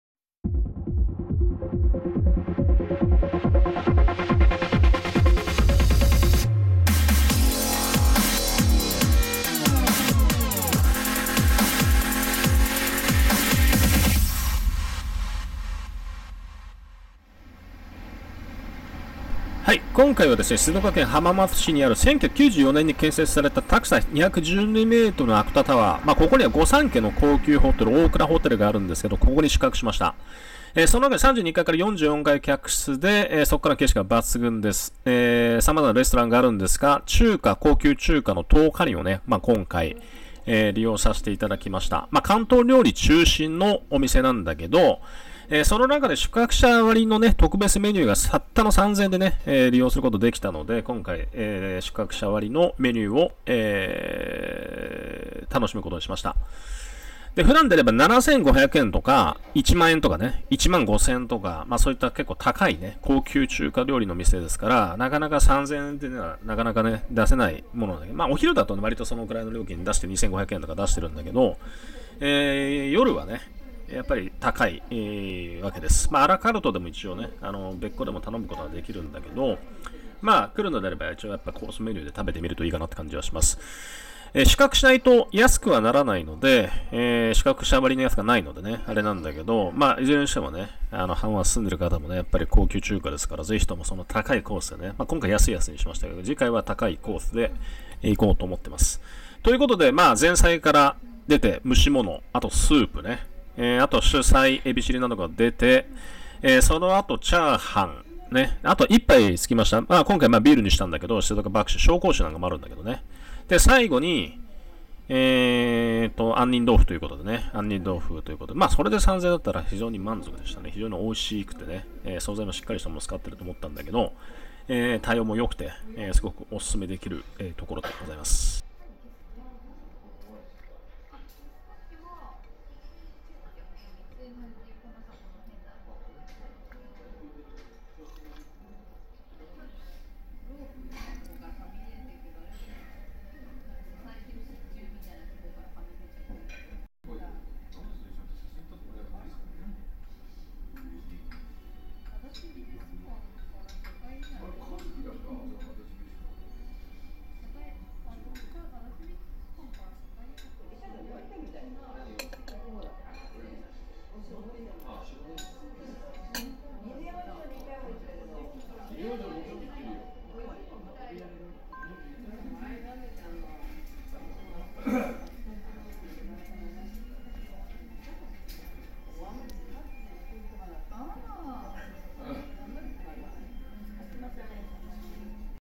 浜松市のお薦め高級中華料理屋「桃花林」御三家のオークラホテル浜松内！アクトシティ(タワー)にあるお薦め店で宿泊者割りコース！Tohkarin Chinese food,Hamamatsu city
御三家のホテルオークラ、アクトシティホテルのレストランを久しぶりに利用。
新型コロナの影響で、複数のレストランは閉まっていし、LOの時間が19:30。
少しLOの時間が過ぎて閉まったが、レストランの配慮で入店出来ました。
オークラホテル内の
高級中華料理のお店に。